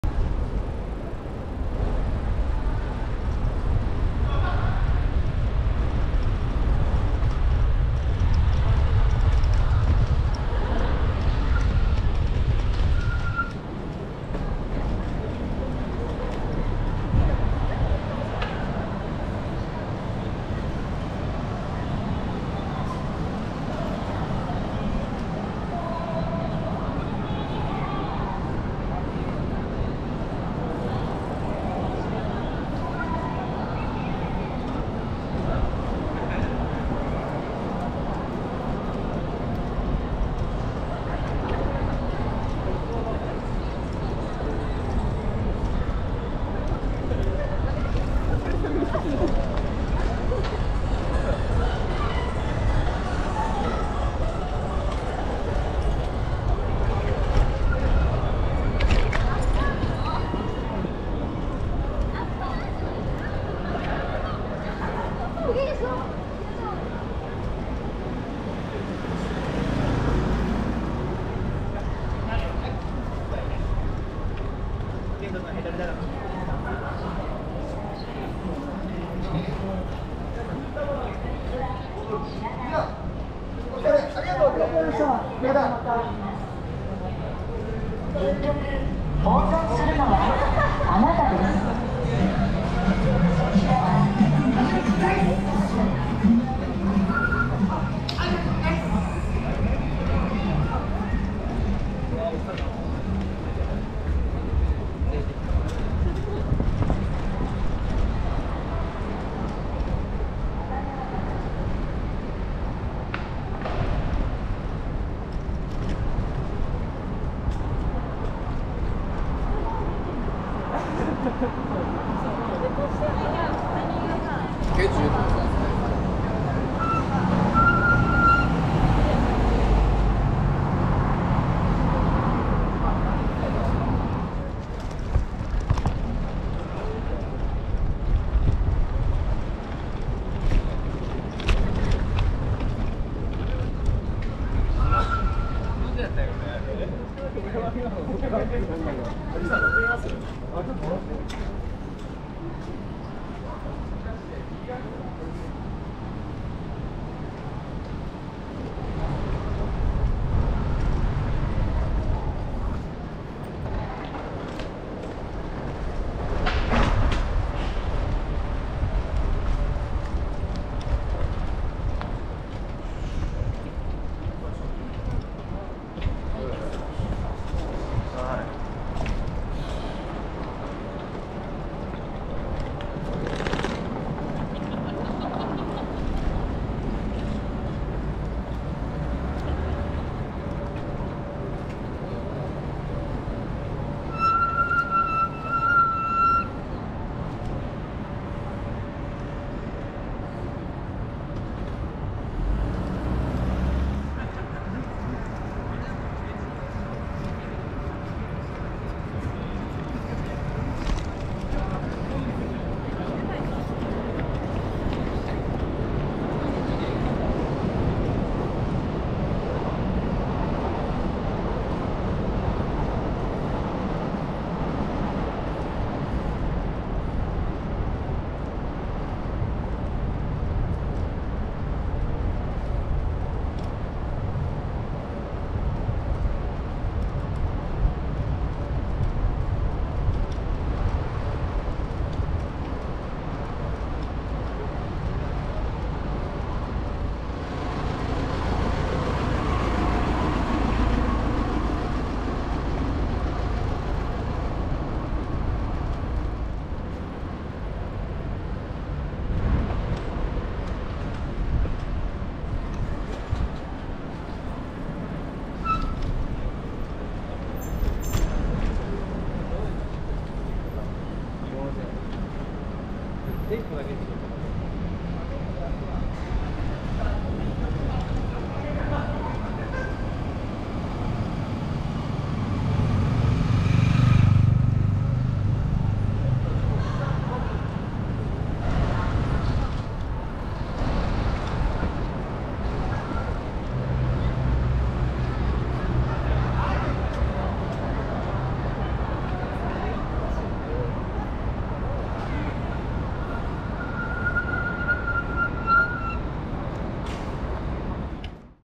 歌舞伎町から新宿駅南口
5月下旬の日曜23時頃の様子